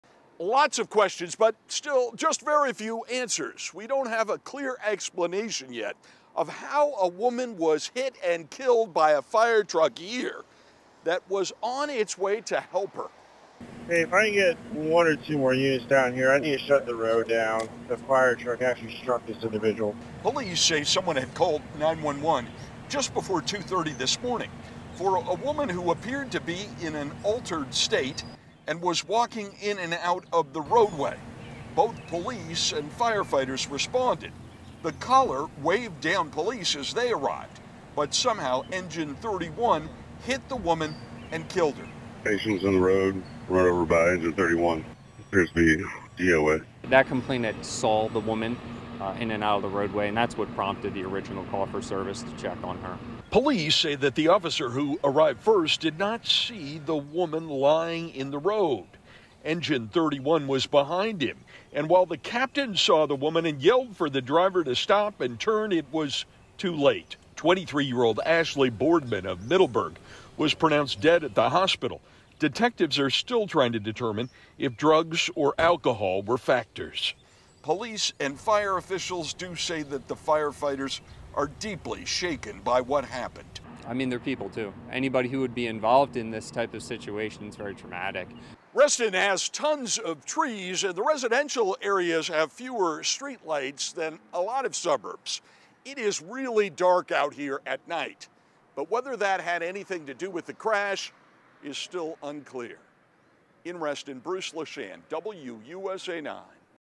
Firetruck hits and kills woman it was on it's way to help in Virginia
New details in the past hour about how a fire truck ended up hitting and killing a woman in Fairfax County.